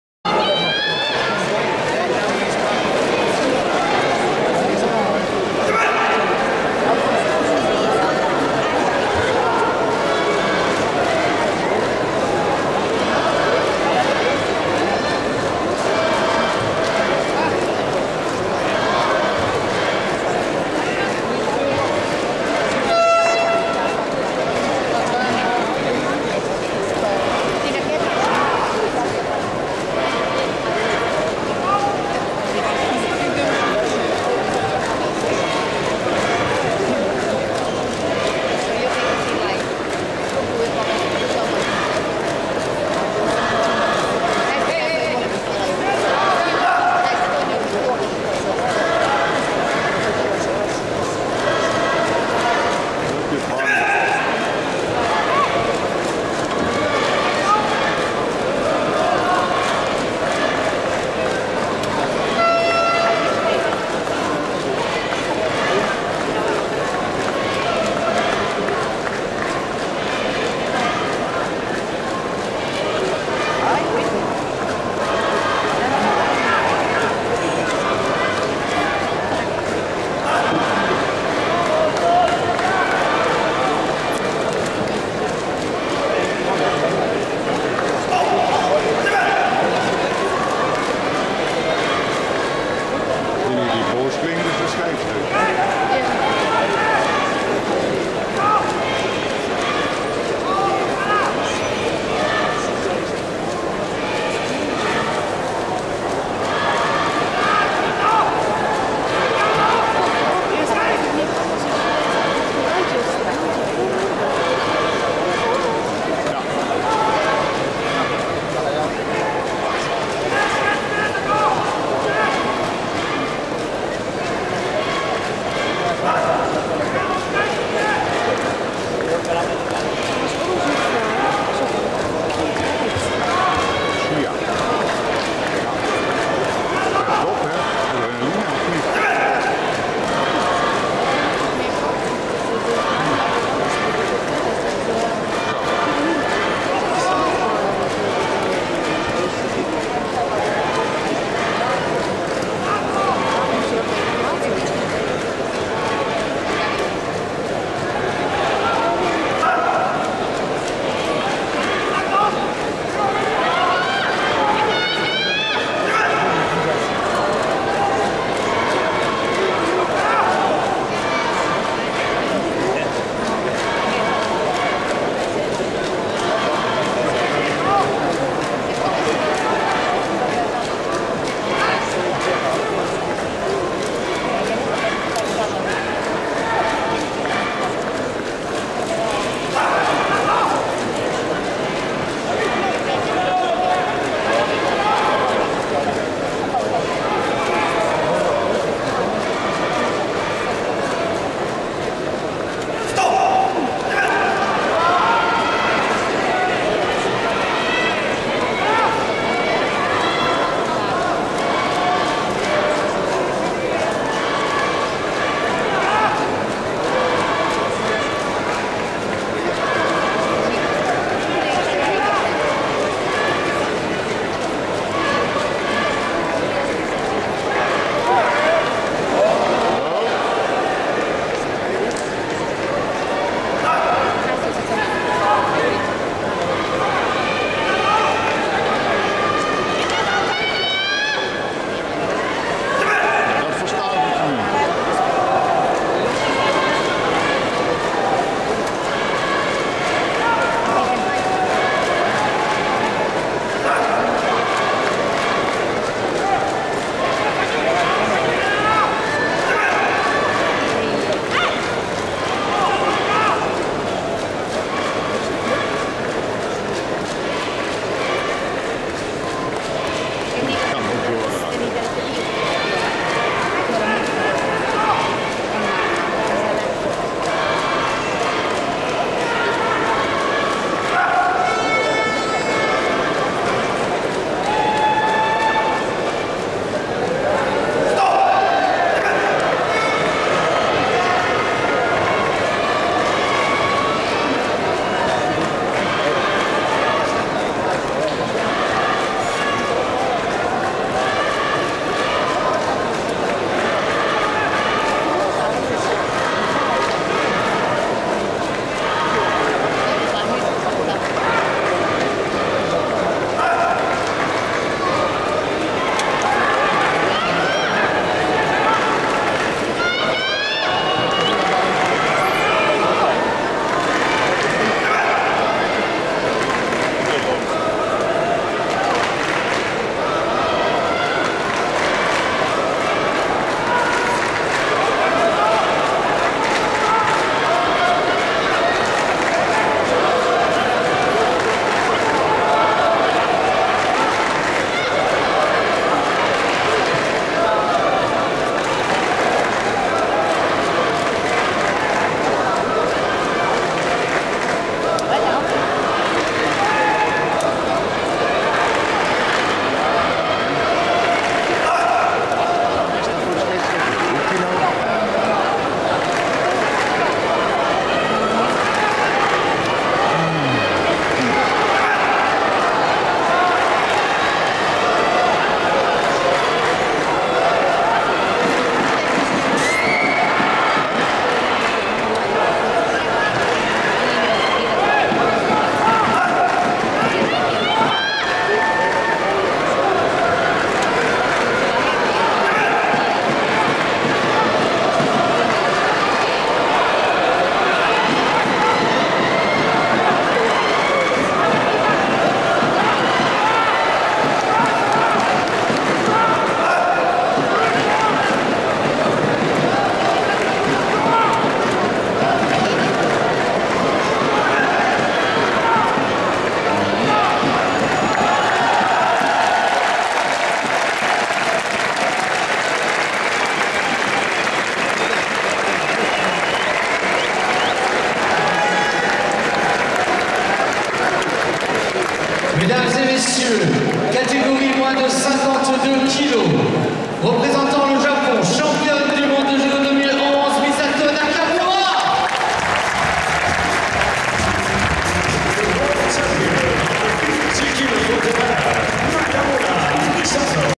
Judo 2011 World Championships Paris: Nishida (JPN) - Nakamura (JPN) [-52kg] final
Yuka Nishida (JPN) - Misato Nakamura (JPN) [-52kg] final HD
Judo 2011 World Championships Paris (private recording).
The only judo-video site just for fun, no commercials